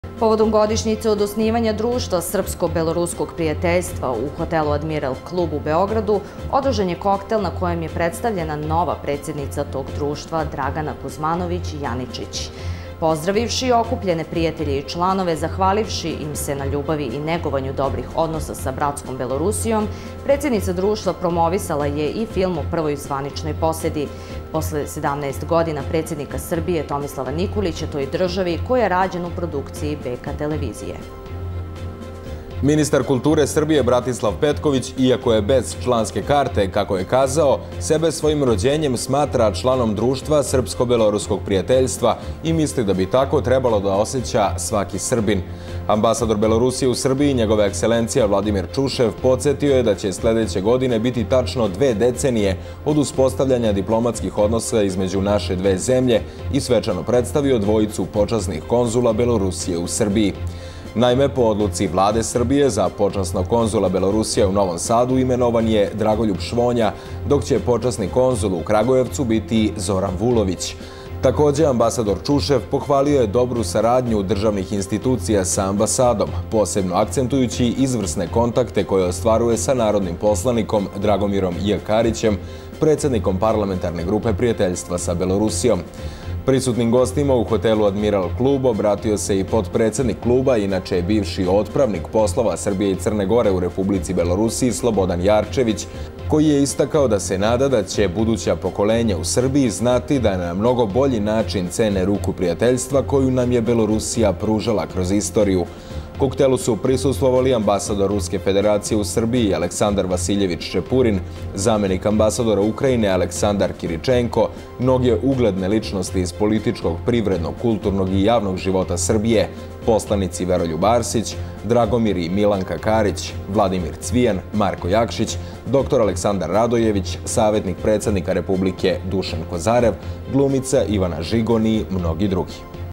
Godišnjica Društva srpsko -- beloruskog prijateljstva 4.4.2013.
Povodom godišnjice od osnivanja Društva srpsko -- beloruskog prijateljstva u Hotelu „ Admiral klub" u Beogradu održan je koktel na kojem je predstavljena nova predsednica Dragana Kuzmanović-Janičić.

Pozdravivši okupljene prijatelje i članove, zahvalivši im na ljubavi i negovanju dobrih odnosa sa bratskom Belorusijom, predsednica društva promovisala je i film o prvoj zvaničnoj poseti, posle 17 godina, predsednika Srbije Tomislava Nikolića toj državi koji je rađen u produkciji BK Televizije.

Ministar kulture Srbije Bratislav Petković iako je bez članske karte, kako je kazao, sebe svojim rođenjem smatra članom društva i misli da bi tako trebalo da oseća svaki Srbin.

Ambasador Belorusije u Srbiji, Njegova ekselencija Vladimir Čušev poželevši uspeha u radu, podestio je da će sledeće godine biti tačno 20 godina od uspostavljanja diplomatskih odnosa između naše dve zemlje, svečano predstavivši dvojicu počasnih konzula Belorusije u Srbiji. Naime, po odluci vlade Srbije za počasnog konzula Belorusije u Novom Sadu imenovan je Dragoljub Švonja dok će počasani konzul u Kragujevcu biti Zoran Vulović. Ambasador Čušev pohvalio je dobru saradnju državnih institucija sa ambasadom posebno akcentujući izvrsne kontakte koje ostvaruje sa narodnim poslanikom Dragomirom J. Karićem, predsednikom Parlamentarne grupe prijateljstva sa Belorusijom.

Prisutnim gostima u Hotelu „ Admiral klub" obratio se i potpredsednik kluba inače, bivši Otpravnik poslova SCG u Republici Belorusiji Slobodan Jarčević. On je rekao da se nada da će buduća pokolenja u Srbiji znati da na mnogo bolji način cene ruku prijateljstva koju nam je Belorusija pružala kroz istoriju.